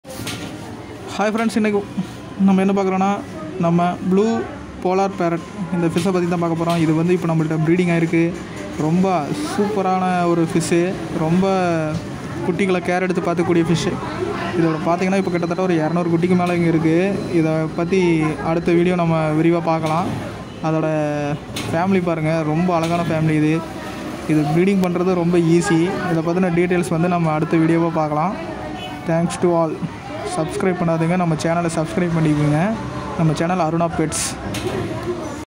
PolarParrotFishBreedinginTamil shorts
hi friends blue polar parrot fish breeding in Tamil
This fish is very easy to breeding and very aggressive and polar parrot fish very good parents for product fryers.